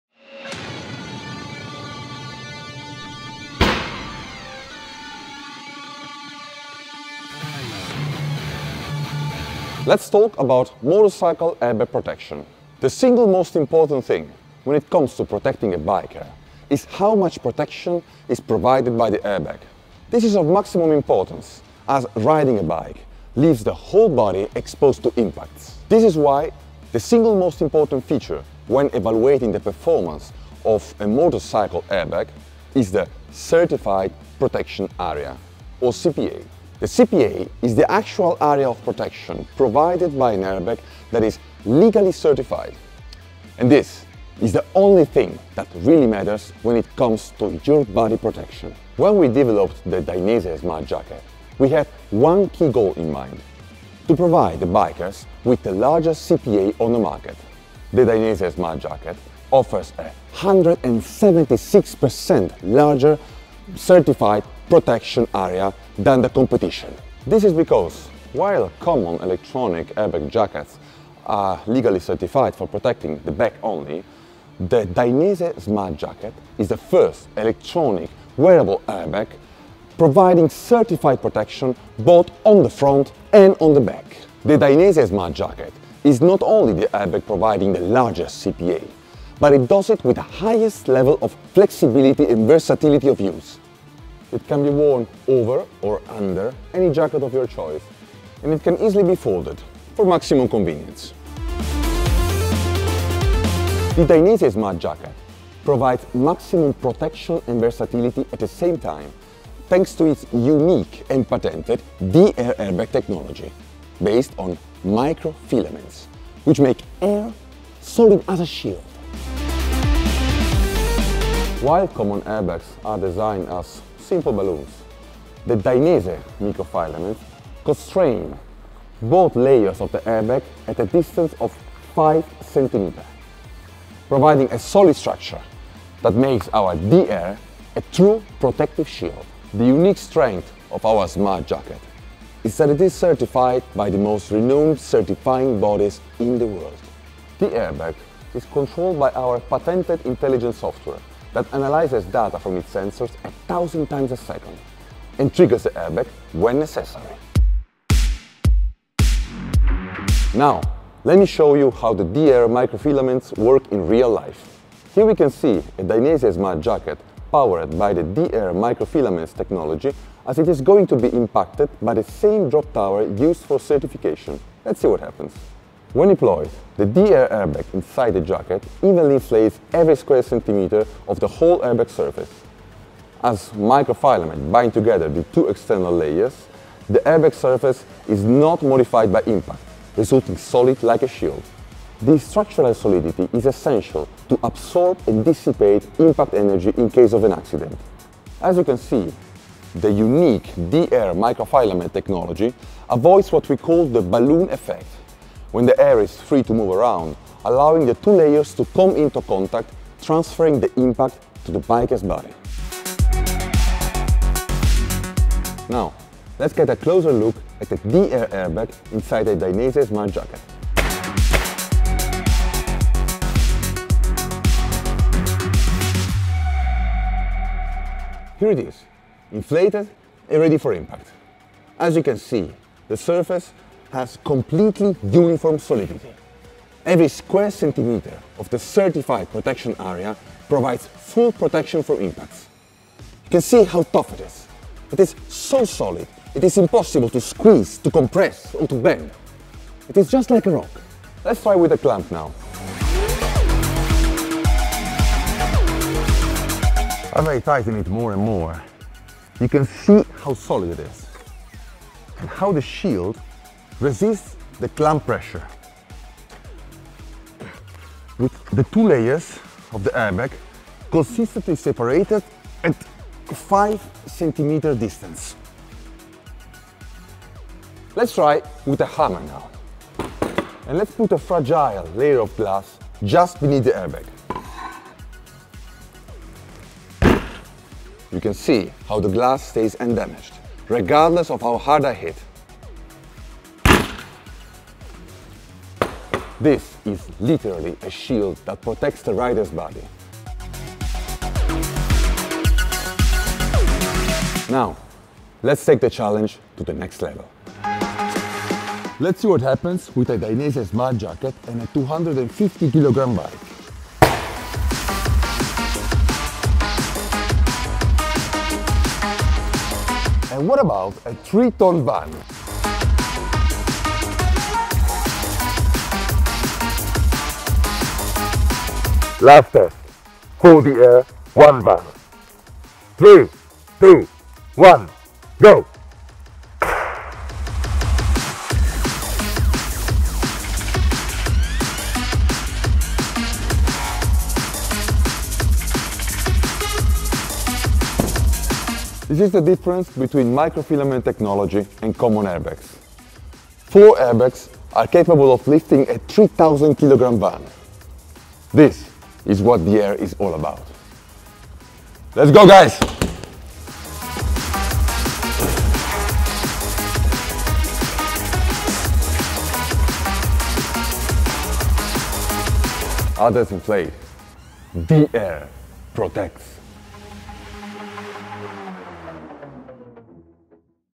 Things you should know about motorcycle airbags | The Smart Jacket | Dainese
How Dainese Smart Jacket stands out from other motorcycle airbags?

When Dainese invented the first electronically-activated airbag for road motorcycles, it had only one goal in mind: To offer the best possible protection in a garment that is comfortable and usable by everyone.

Years have passed since then, during which the R&D department has continued to work tirelessly, constantly raising the bar. Today, we are able to offer you Dainese Smart Jacket, a comfortable, lightweight and versatile vest that, owing to the innovative patented D-air® technology, additionally offers incredible certified protection, higher than that of traditional motorcycle airbags.

Let's see together in this video how Dainese D-air® airbag works and what are the most important things you should know when evaluating the effective protection of a motorcycle airbag.